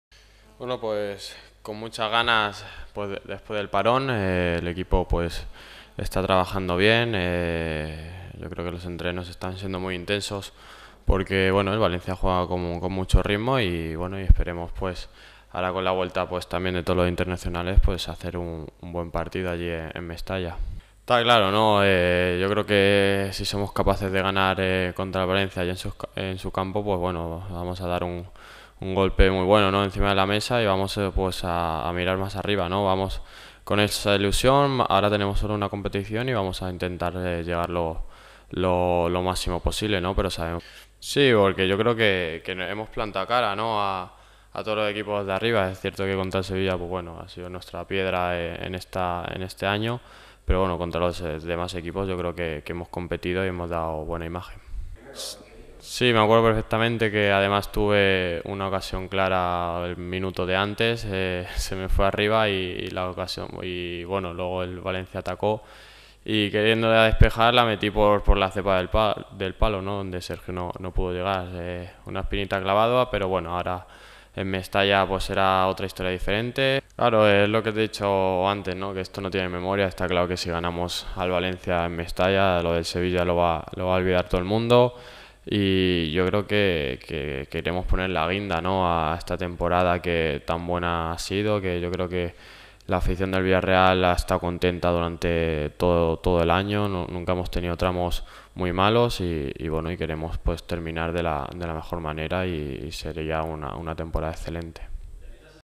Manu Trigueros: "Queremos terminar de la mejor manera"
El futbolista del Villarreal CF habla ante los medios de comunicación días antes del derbi ante el Valencia.